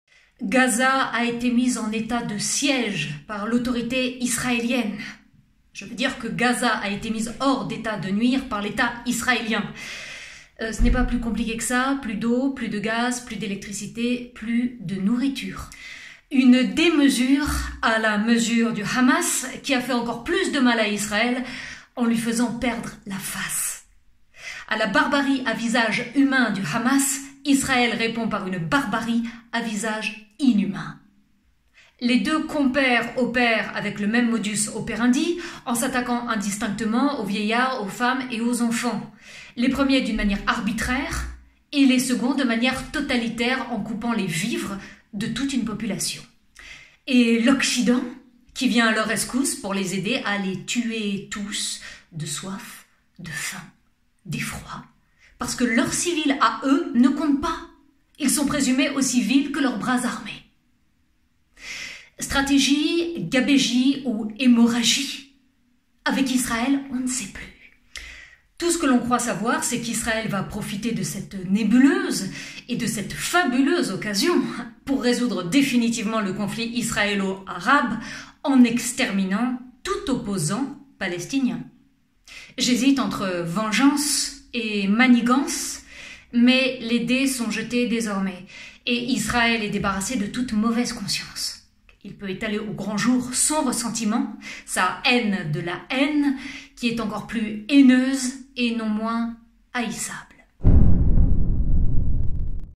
GAZA EN ÉTAT DE SIÈGE !
Gaza a été mise en état de siège par l’autorité israélienne, je veux dire que Gaza a été mise hors d’état de nuire par l’État Israélien…
Ce n’est pas plus compliqué que ça : plus d’eau, plus de gaz, plus d’électricité, plus de nourriture.

Une démesure à la mesure du Hamas qui a fait encore plus de mal à Israël en lui faisant perdre la face.
A la barbarie à visage humain du Hamas, Israël répond par une barbarie à visage inhumain.

Les deux compères opèrent avec le même « modus operandi » en s’attaquant indistinctement aux vieillards, aux femmes et aux enfants.
Les premiers d’une manière arbitraire et les seconds de manière totalitaire en coupant les vivres de toute une population.

Et l’Occident qui vient à leur rescousse pour les aider à les tuer tous… de soif, de faim et d’effroi. Parce que leurs civils à eux ne comptent pas, ils sont présumés aussi vils que leurs bras armés.
Stratégie, gabegie ou hémorragie ? Avec Israël, on ne sait plus…

Tout ce que l’on croit savoir, c’est qu’Israël va profiter de cette nébuleuse ou fabuleuse occasion pour résoudre définitivement le conflit israélo-arabe en exterminant tout opposant palestinien.

J’hésite entre vengeance et manigance, mais les dés sont jetés désormais et Israël est débarrassé de toute mauvaise conscience, il peut étaler au grand jour son ressentiment… sa haine de la haine… qui est encore plus haineuse et non moins haïssable.